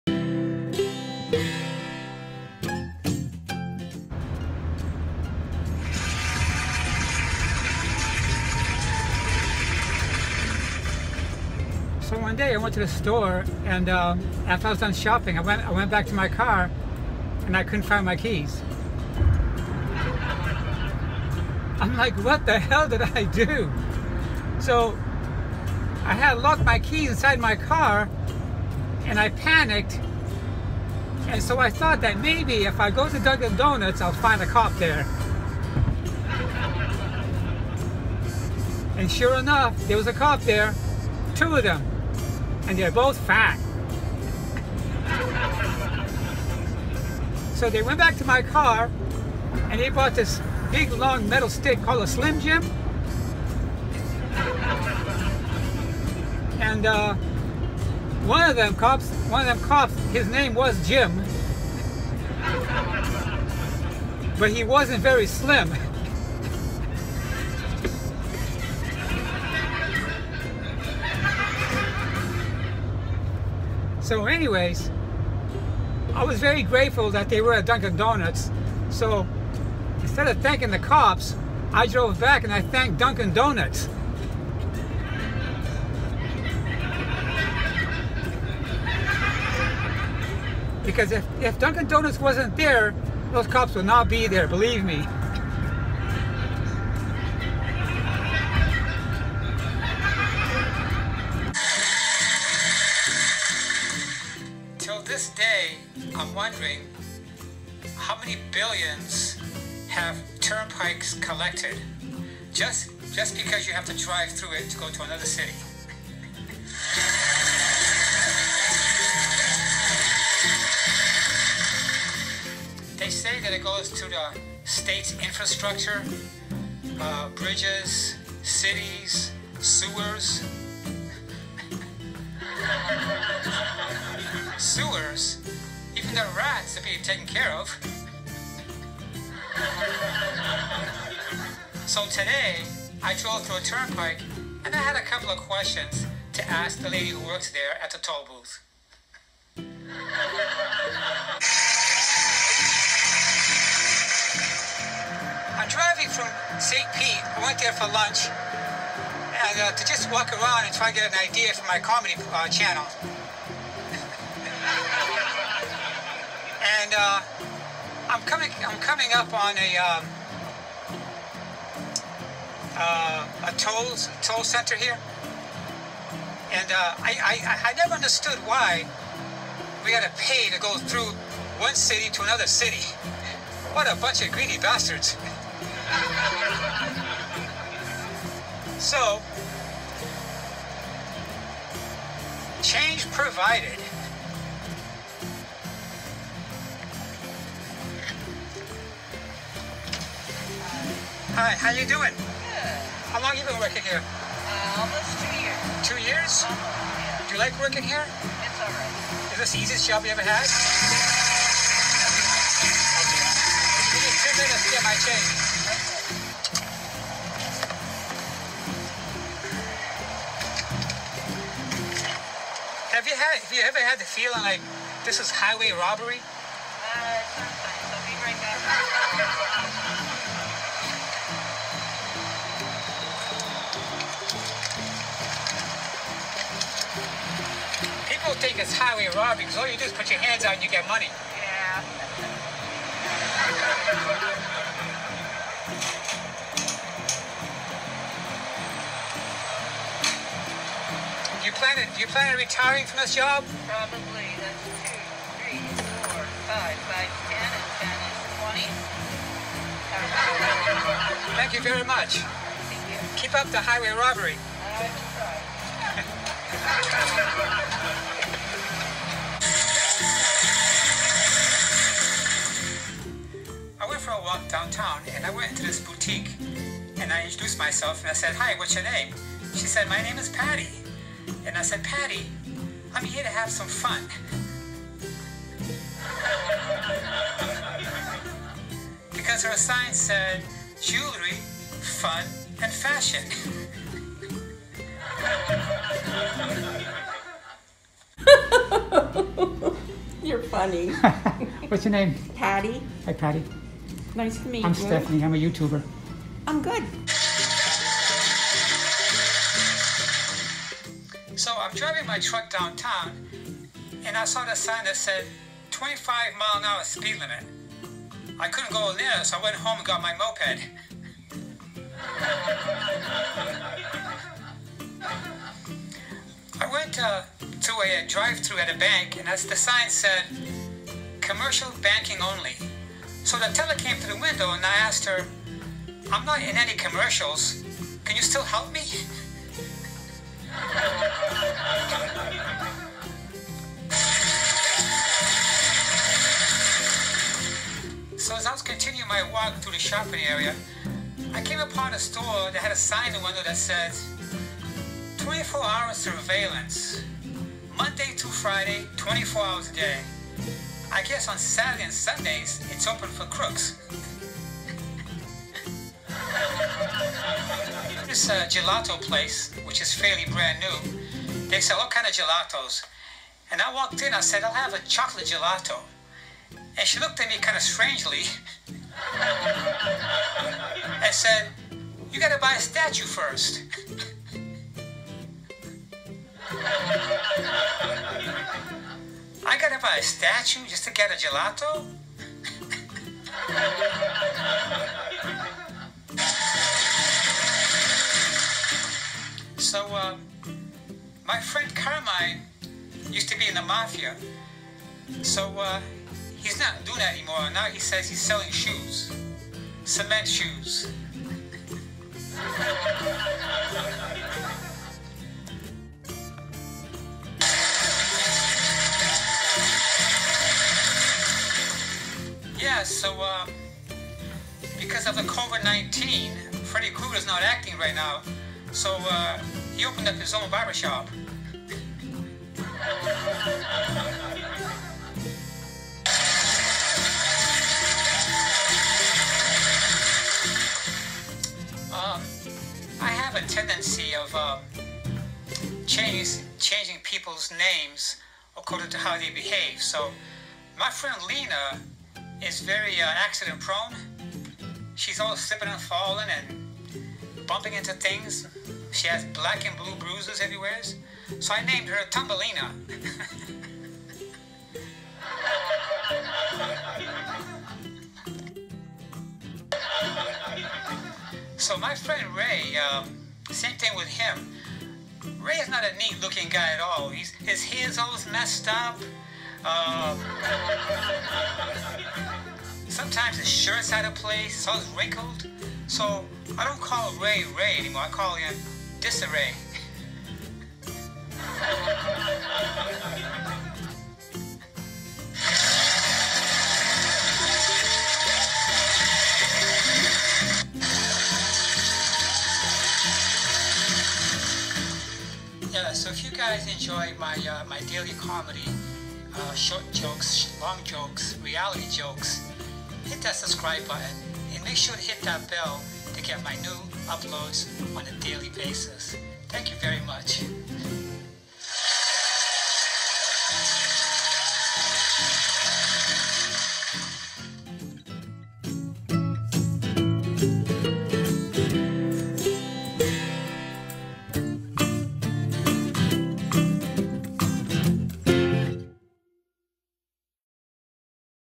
Paying Tolls
Episode 23 Driving through Florida turnpike.